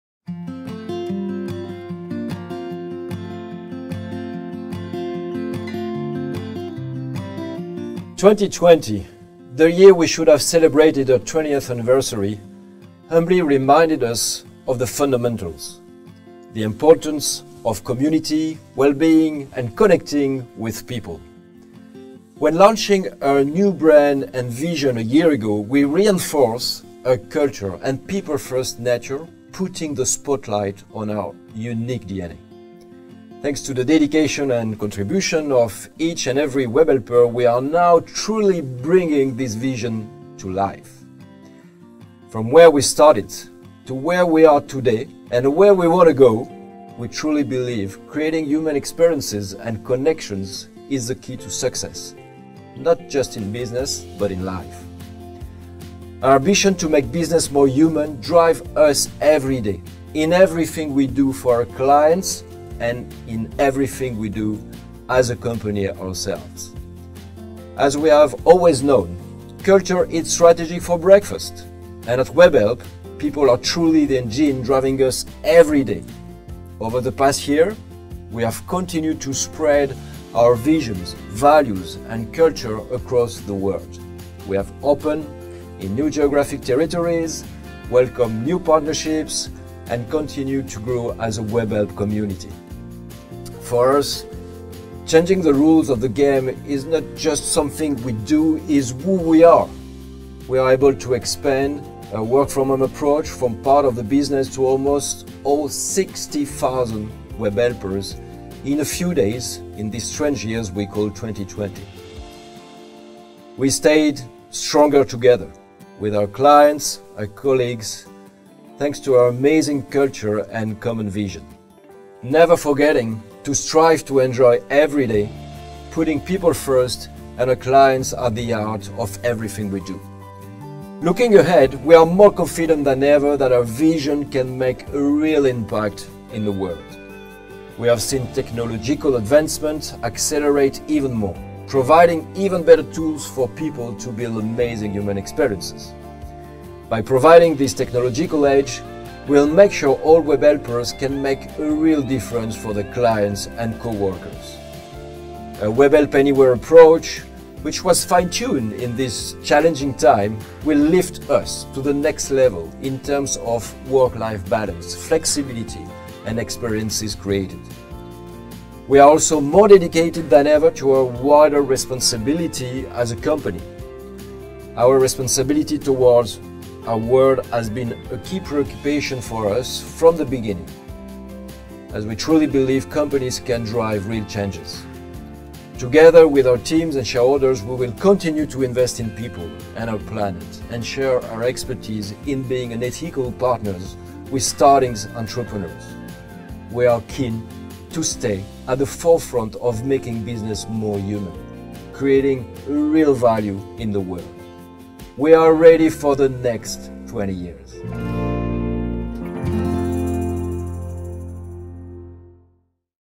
Making business more human…propelling us forward!
It's been a great week looking back at how we make business more human every day. Now let's hear from our co-founder Olivier how our vision drives our ambitions for the future… on to the next 20 years!